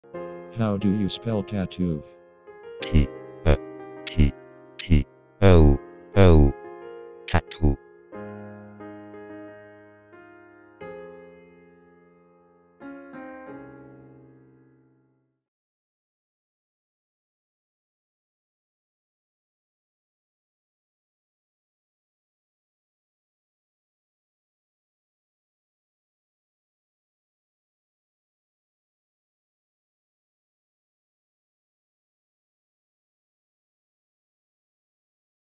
How do you spell tattoo?
Learn how to spell and pronounce tattoo.